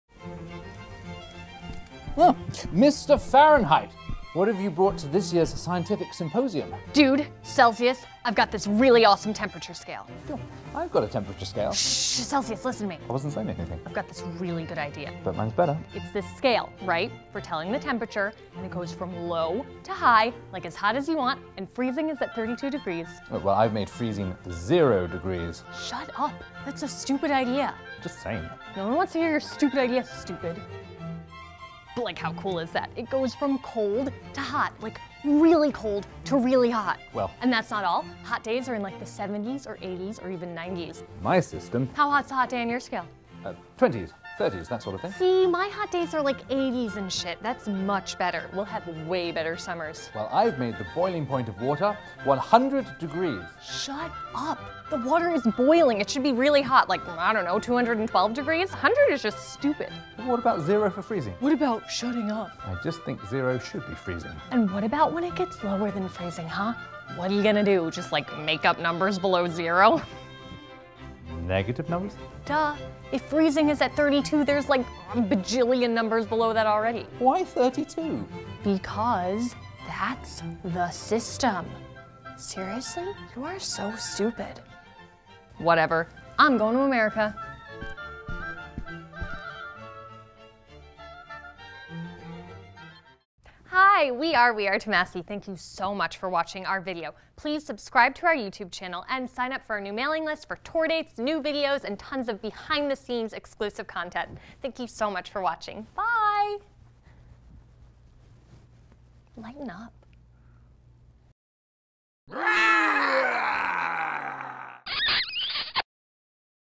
WHAT THE HISTORY! - Fahrenheit vs Celsius
Boy oh boy is it getting hot in here! When Fahrenheit and Celsius go head to head, things come to a boil as these two men get the measure of each other. Will Celsius freeze Fahrenheit out? Will Fahrenheit give Celsius the cold shoulder? Watch and find out!

Starring: Nick Afka Thomas and Sarah Ann Masse
Written by Sarah Ann Masse and Nick Afka Thomas (We Are Thomasse)
Directed by Ed Ballart
Sound by Nico Pierce
Edited by Ed Ballart
Hair and Makeup by Gabrielle Rossbach
Produced by Zonino Productions & We Are Thomasse (Sarah Ann Thomas)

Special Thanks to Ruth Jervis, Ed Toolis and Charles and Enid Humfrey.